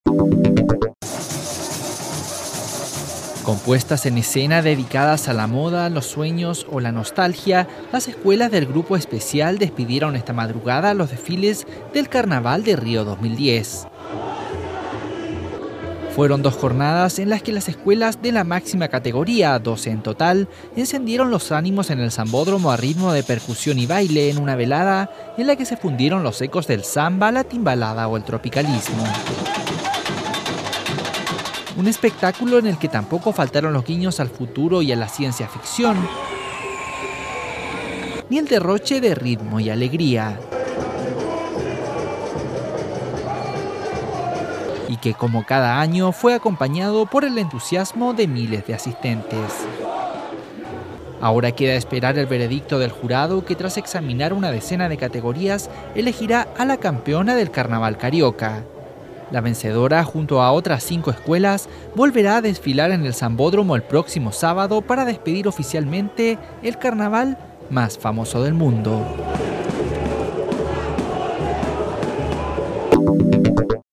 Las escuelas de samba despiden el carnaval de Río
Rio de Janeiro (Brasil), 16 feb (EFE), (Imágenes: Elisa Martins).- Las escuelas de samba de Río de Janeiro pusieron en la madrugada de hoy punto final a dos jornadas de desfiles del carnaval y cuya apertura estuvo a cargo de Mocidade Independiente de Padre Miguel con una representación sobre los paraísos.